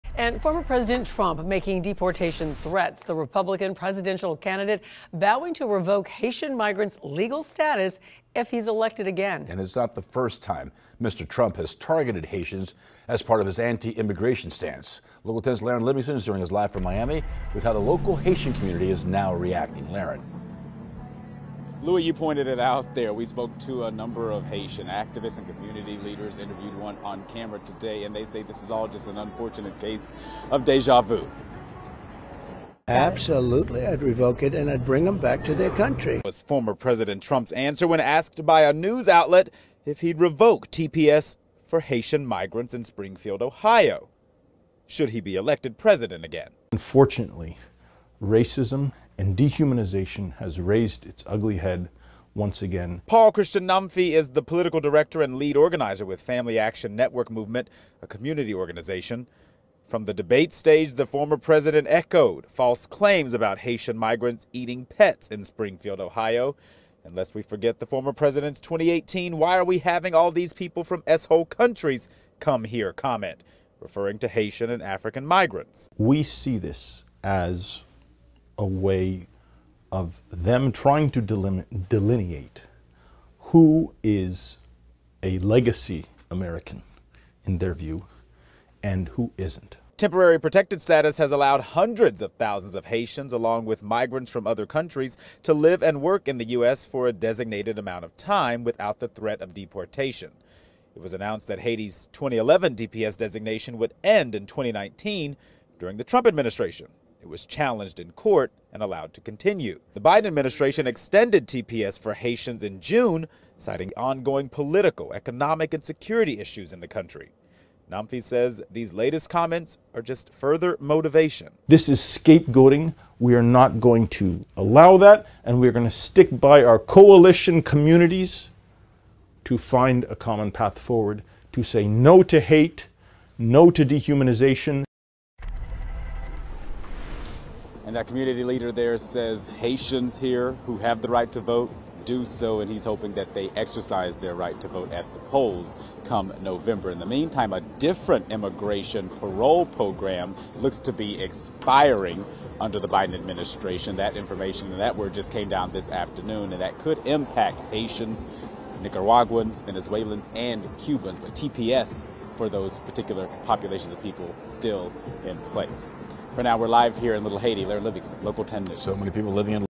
November election concerns Haitian migrants, advocates
Former President Donald Trump has voiced his desire to get rid of the Temporary Protected Status for Haitians so many times that migrants and advocates are worried that he may get reelected this November.
LOCAL10 NEWS on YouTube: YouTube.com/@wplglocal10